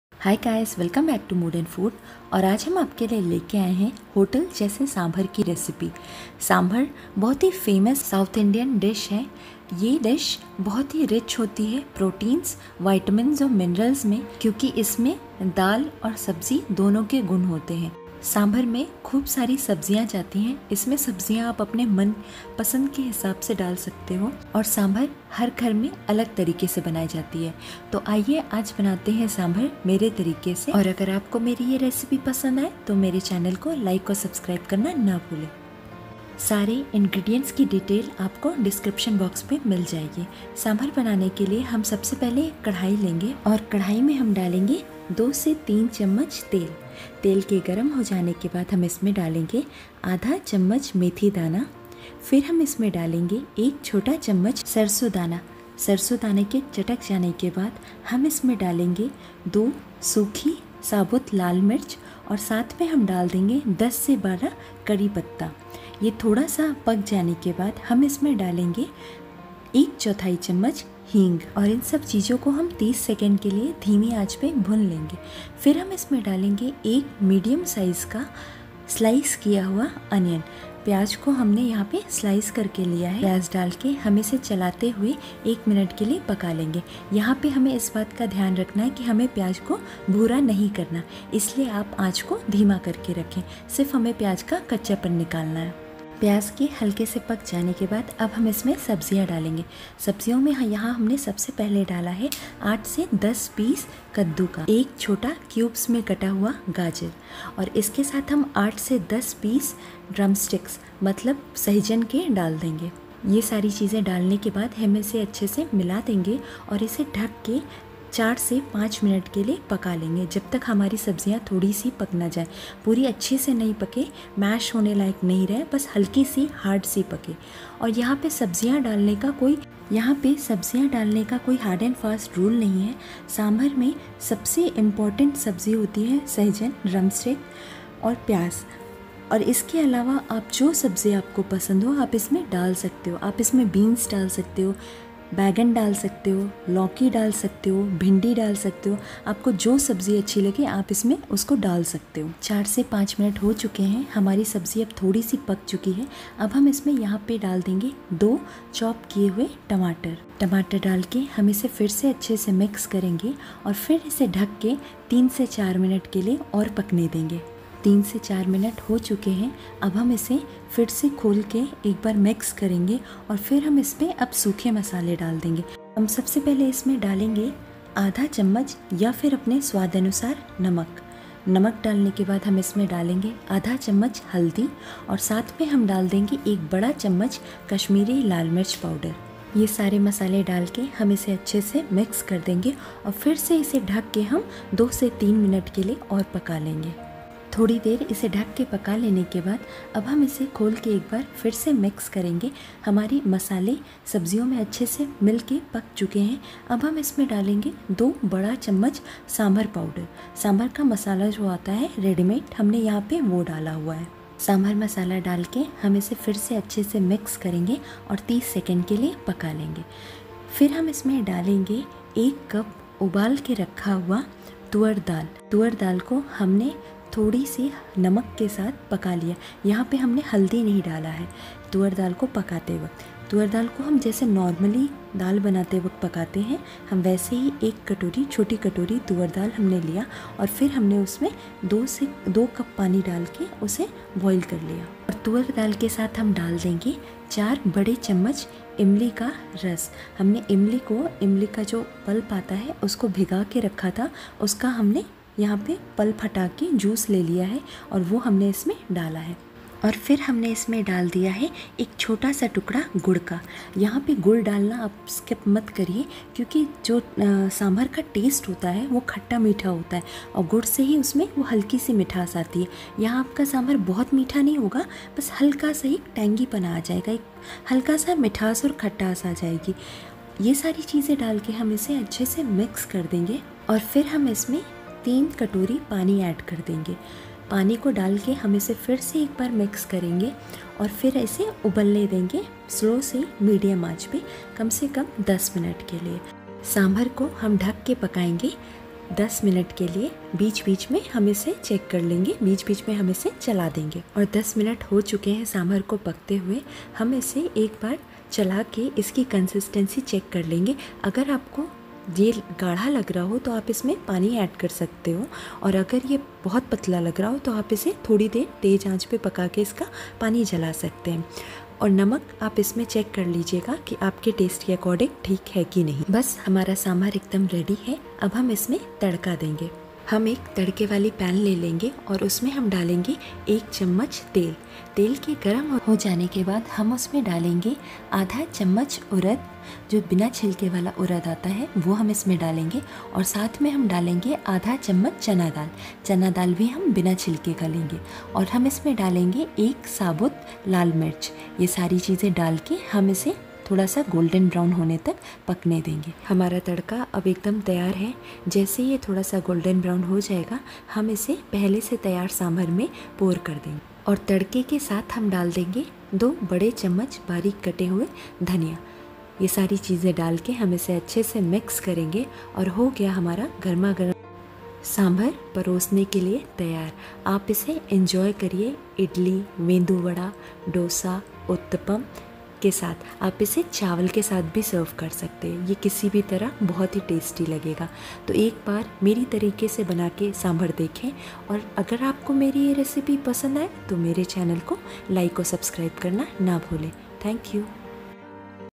होटल जैसी टेस्टी सांभर घर पर बनाने का आसान तरीका | Hotel Style Sambhar Recipe | South Indian Sambar
Sambar, a hot and spicy lentil soup with mixed vegetables, is a must have accompaniment for popular South Indian breakfast and lunch recipes like Idli, Dosa, Medu Vada, Steamed Rice etc.
sambar or sambhar is a lentil based stew mixed with vegetable and tamarind juice. once the lentil soup comes to a boil, it is then mixed with spice powder known as sambar powder. also, sambar is incomplete without vegetables. while any vegetables can be added to sambar. however, drumstick is a must in any south indian sambar recipe.
‎@Kabita's Kitchen  ‎@Hebbars Kitchen  ‎@Chef Ranveer Brar  ‎@Cook With Parul

Ingredeints:-
• 150 grm boiled arahar/tuar daal
• 2 tablespoon oil
• One tsp mustard seed
• 1/2 tsp  meethi daana
• Few curry leaves
• 2-3 dried red chillies
• 1/2 tsp hing
• 8-10 pieced if drumsticks
• 8-10 pieces of pumpkin
• 1 small carrot diced
• 1 medium size onion sliced
• 2 tomatoes chopped
• 2 tablespoon sambhar masala
• 1/2 tsp haldi
• 1 tsp kashmiri red chilli powder
• Salt according to taste
• 4 tablespoon imli ka pani
• 1 small piece of jaggery

Ingredeints for tempering:-
• 1/2 tsp urad daal
• 1/2 tsp chana daal
• 1-2 dried red chilli